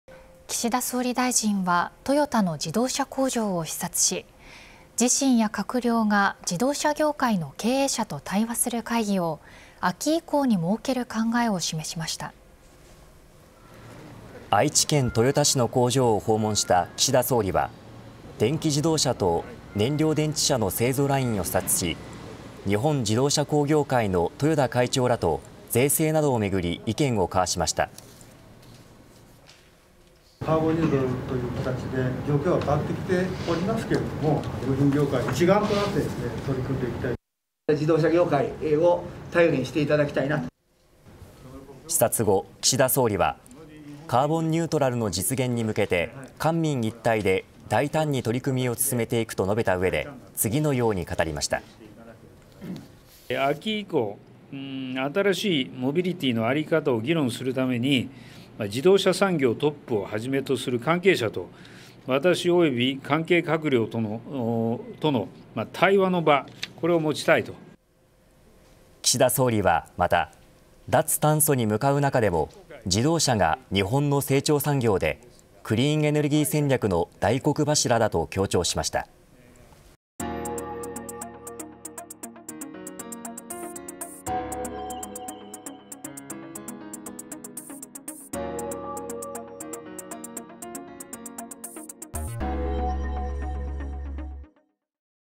岸田首相トヨタ工場へ脱炭素技術を視察 「秋以降に自動車業界対話会議」
岸田首相は、トヨタの自動車工場を視察し、自身や閣僚が自動車業界の経営者と対話する会議を、秋以降に設ける考えを示した。

愛知・豊田市の工場を訪問した岸田首相は、電気自動車と燃料電池車の製造ラインを視察し、日本自動車工業会の豊田会長らと税制などをめぐり、意見を交わした。

自動車部品工業会・有馬会長「カーボンニュートラルという形で状況は変わってきていますが、部品業界一丸となって取り組んでいきたい」

日本自動車工業会・豊田会長「自動車業界を頼りにしていただきたい」

視察後、岸田首相は「カーボンニュートラルの実現に向けて、官民一体で大胆に取り組みを進めていく」と述べたうえで、「秋以降、新しいモビリティーのあり方を議論するために、自動車産業トップをはじめとする関係者とわたし、および関係閣僚との対話の場をもちたい」と語った。

岸田首相はまた、「脱炭素に向かう中でも、自動車が日本の成長産業でクリーンエネルギー戦略の大黒柱だ」と強調した。